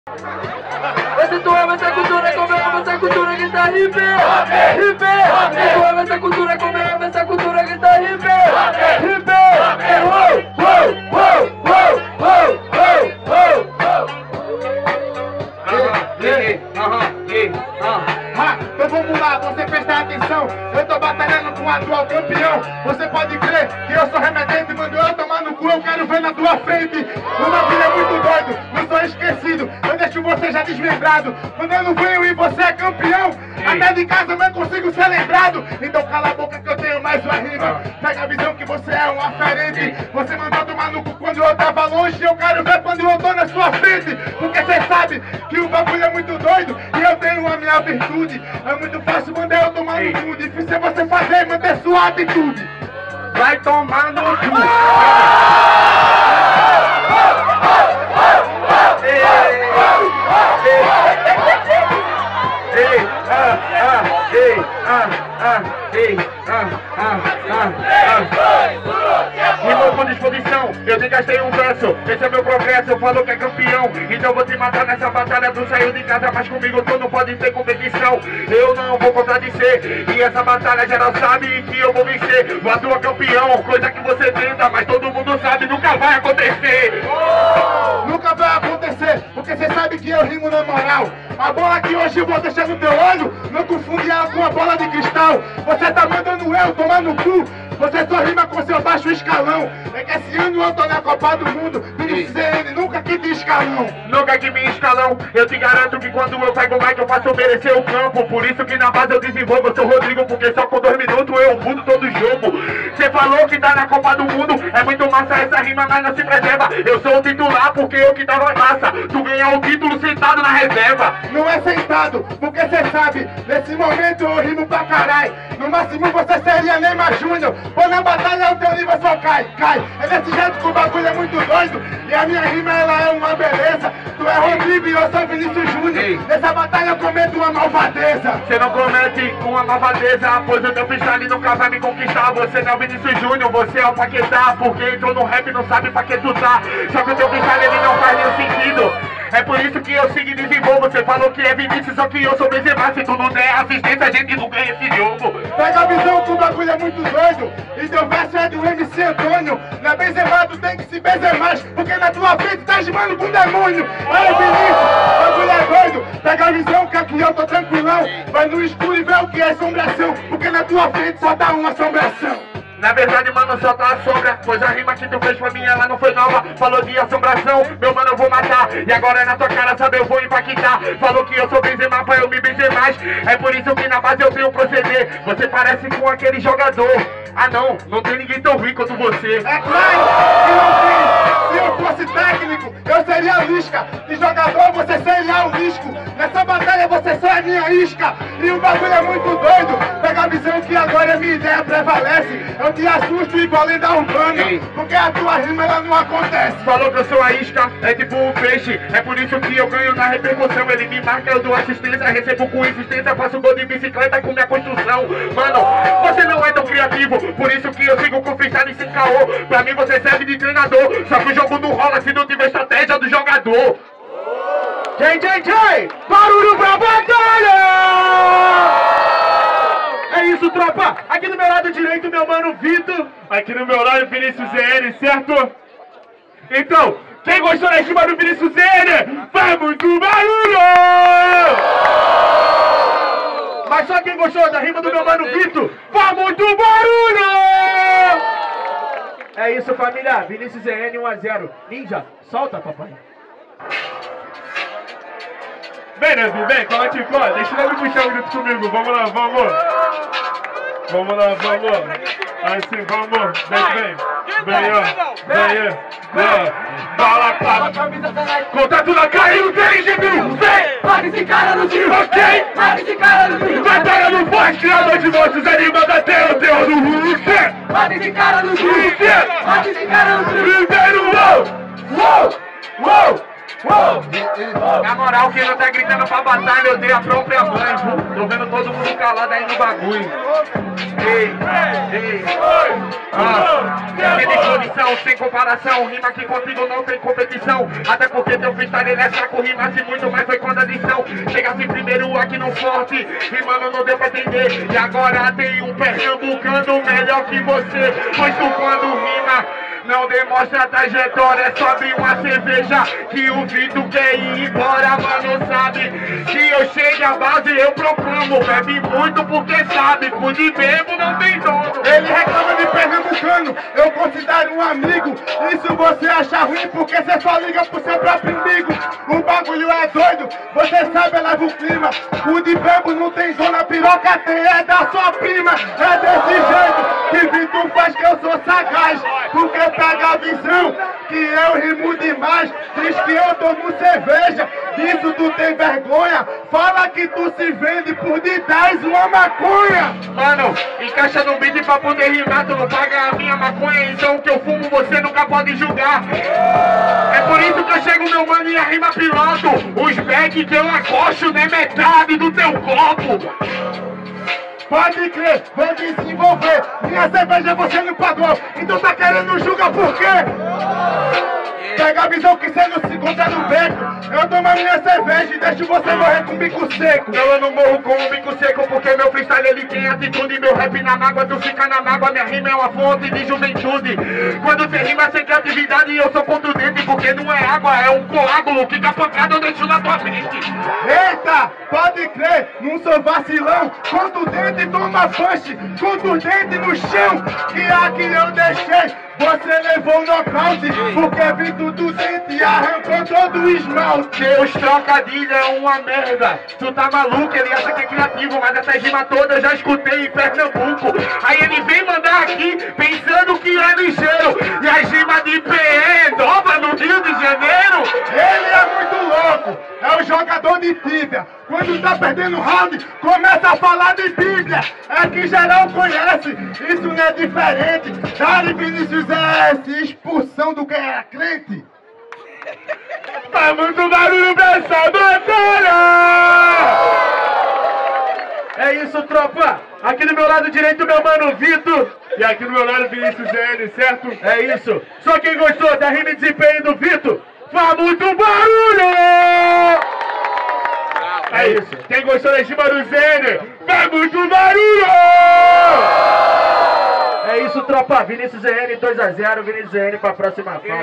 (DUELO DE PERNAMBUCANOS) | VITU x VINICIUS ZN | 1ª FASE | 14ª BATALHA DO FORTE | CABO FRIO | 2022
A Batalha do Forte acontece todas as terças na cidade de Cabo Frio/RJ na praça da cidadania.

TikTok: da Batalha do forte (@batalhadofort)

Filmagem: @rullyver.g